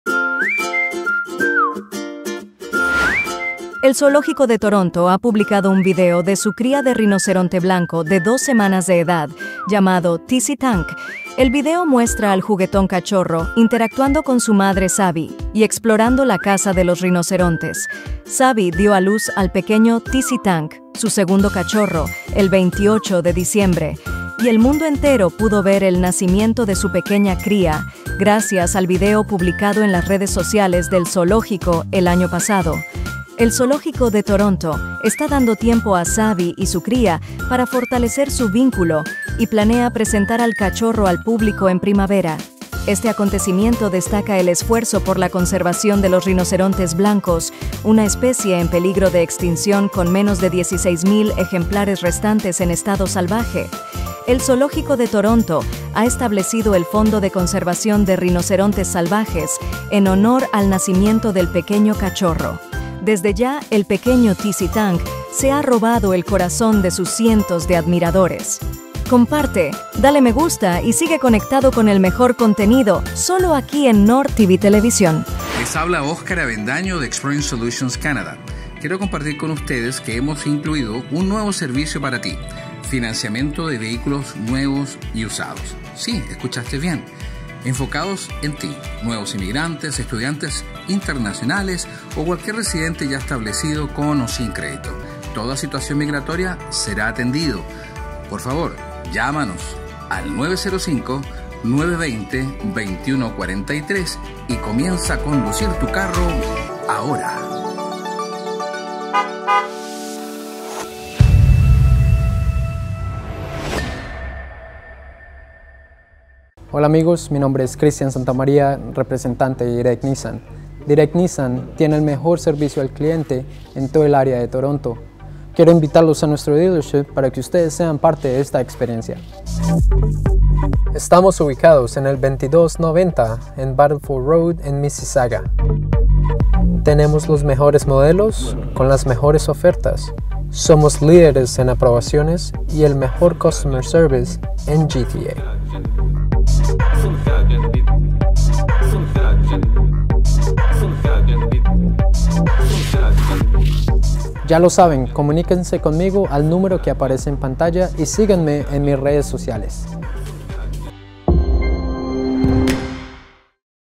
Zoológico de Toronto ha publicado un video de su cría de rinoceronte blanco de dos semanas de edad
El Zoológico de Toronto ha publicado un video de su cría de rinoceronte blanco de dos semanas de edad